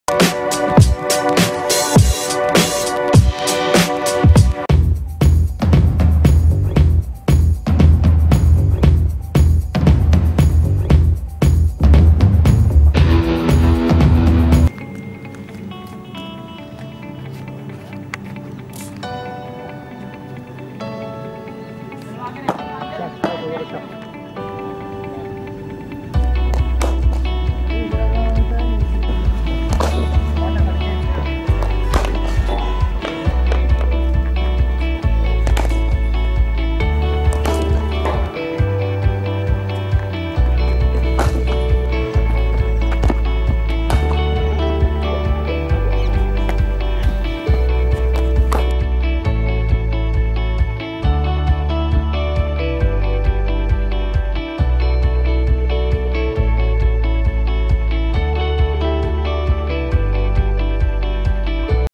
Nikum Saini | The Real Talent | Teaser ! Cricket Zone !
This is the teaser video of upcoming story of 10 years old boy Nikum Saini !